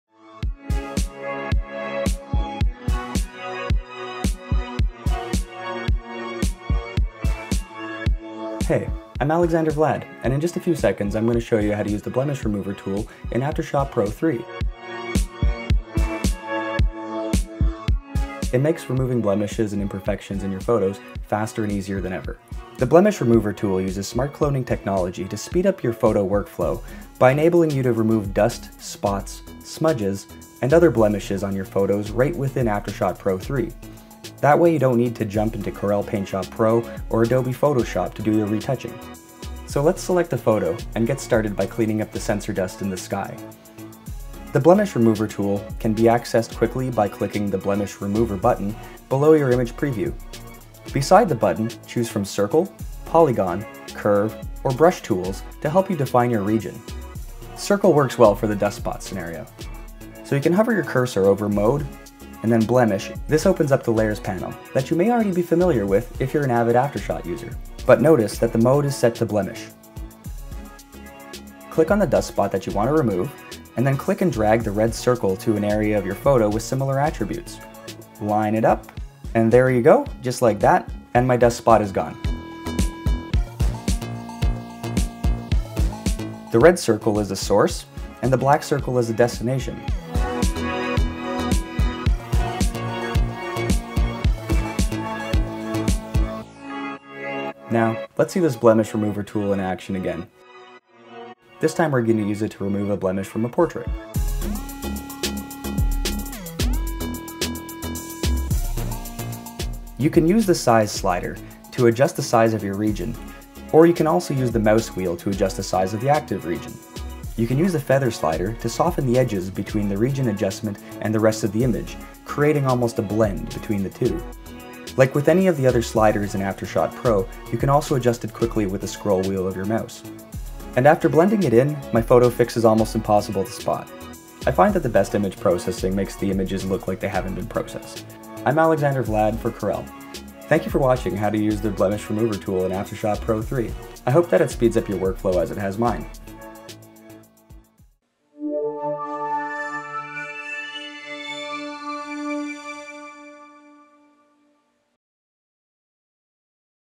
The New Blemish Remover Tool in AfterShot Pro 3
This tutorial will teach you how to use AfterShot Pro 3's new Blemish Remover feature to speed up your photo workflow. The Blemish Remover tool enables you to remove dust, spots, smudges and other blemishes on your photos right within AfterShot Pro 3, so you don’t need to jump into Corel PaintShop Pro or Adobe PhotoShop.